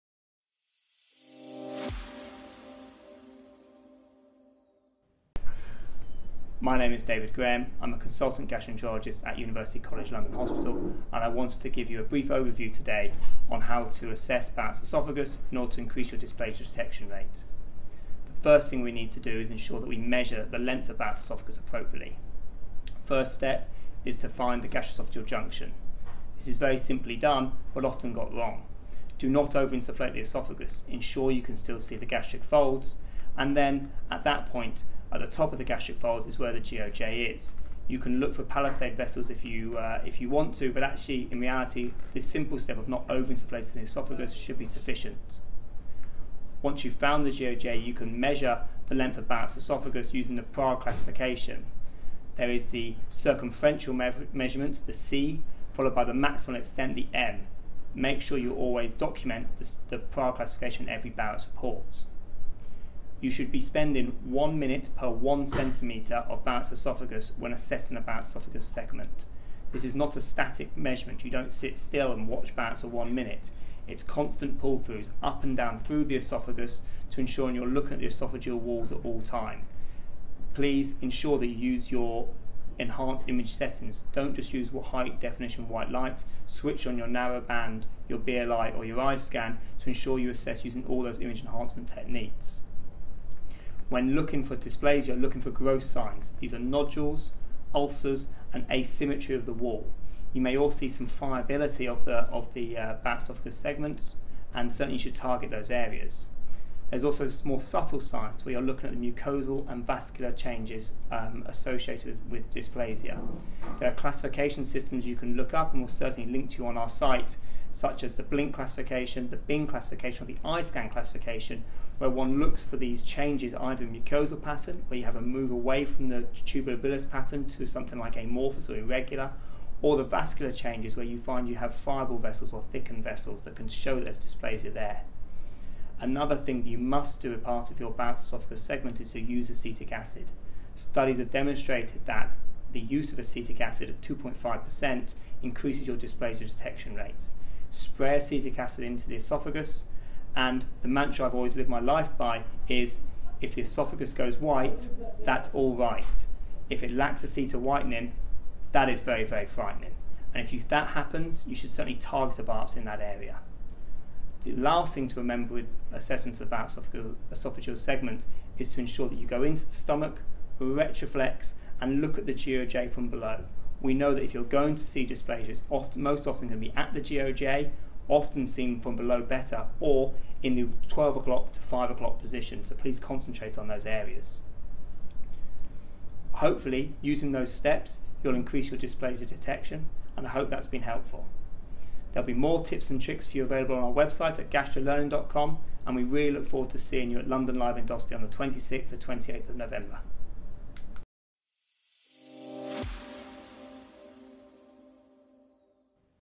Endoscopy Top Tips: UGI - Assessing Barrett's and Increasing Dysplasia Detection
Dr David Graham  - Consultant Gastroenterologist and Upper GI specialist at University College London Hospital (UVCLH) give us his "Top Tips" for assessing Barrett's oesophagus and increasing your dysplasia detection rate. He discusses the imaging classification systems using Pentax iScan  and Fuji BLI technology as well as the use of acetic acid.